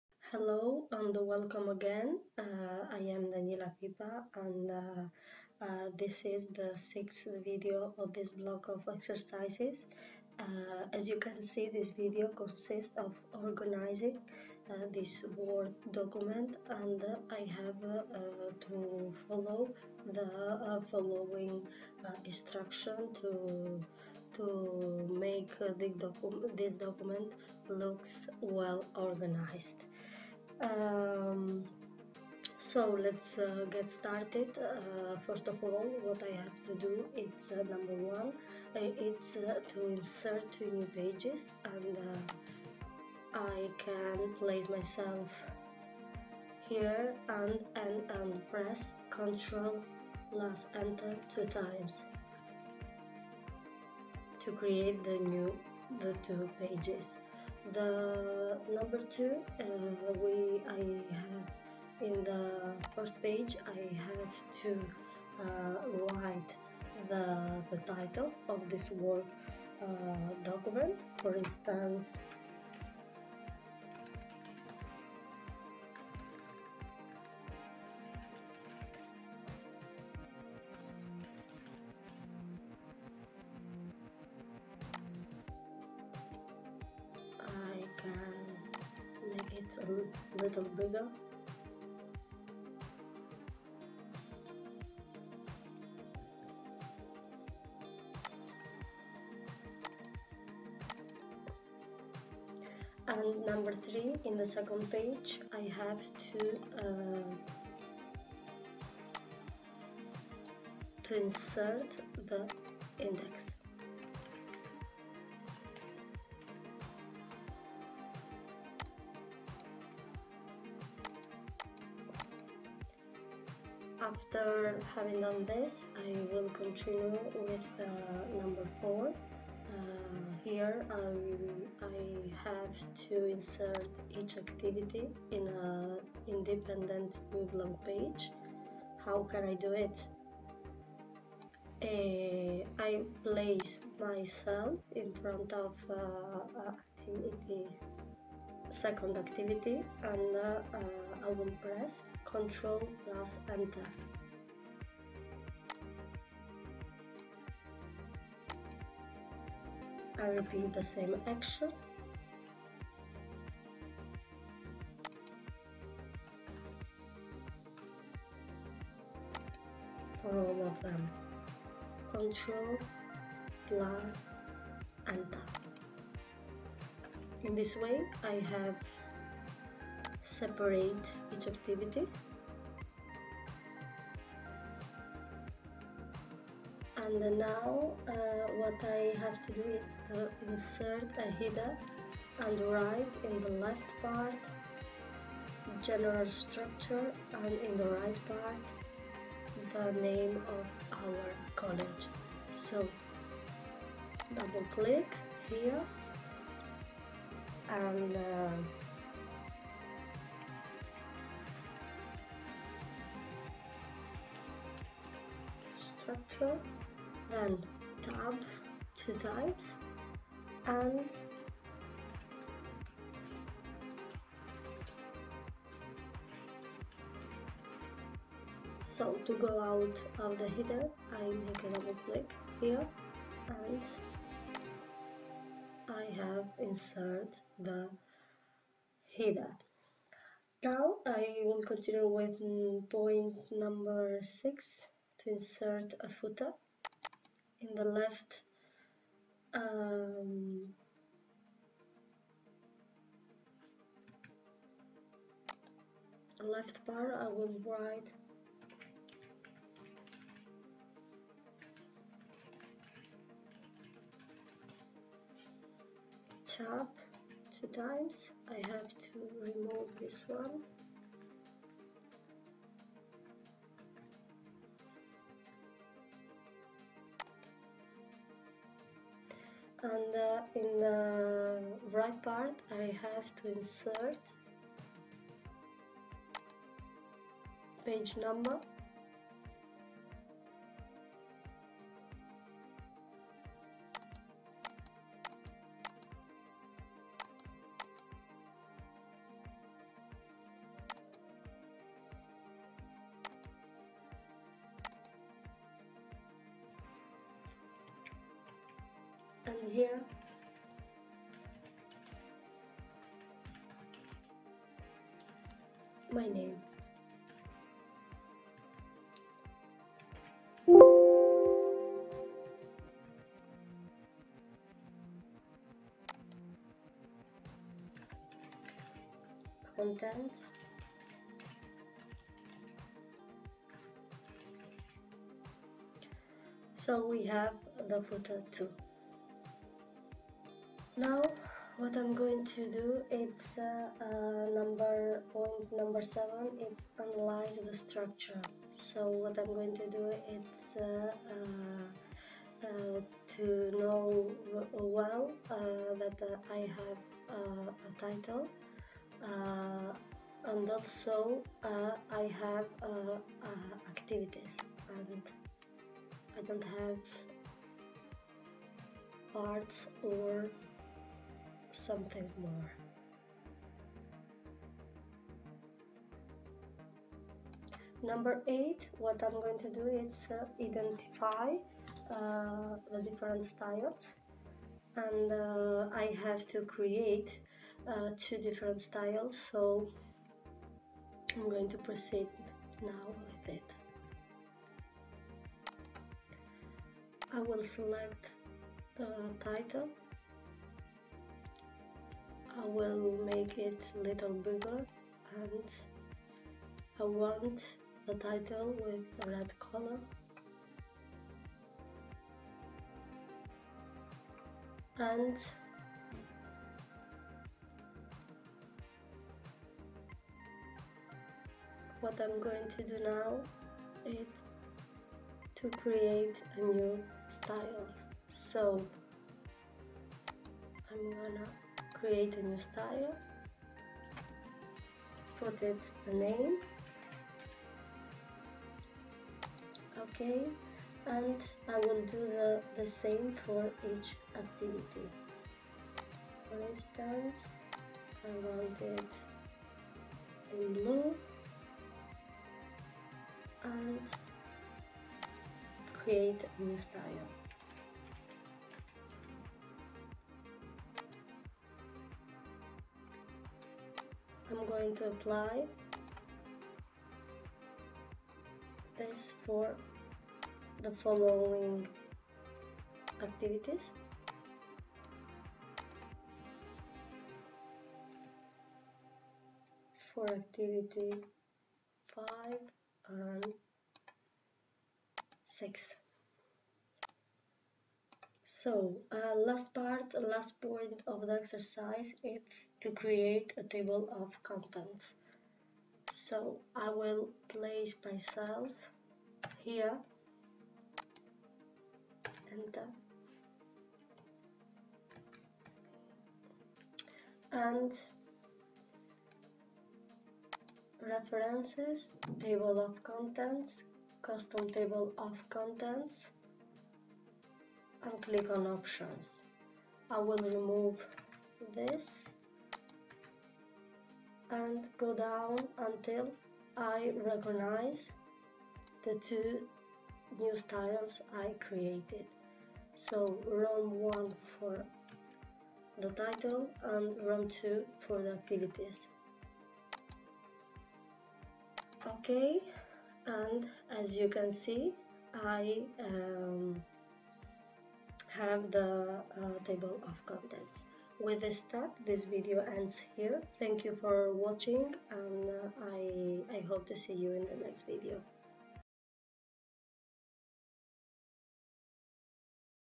06- Toc III
00:35 -Insert two new pages
00:58 -First page-Title
01:42 -Second page-Index
02:08 -Insert each activity in a new blank page
03:10 -Header(left and right part)
05:35 -Footer(left and right part)
05:43 -Analyse the structure
06:17 -Identify styles
08:08 -Create a TOC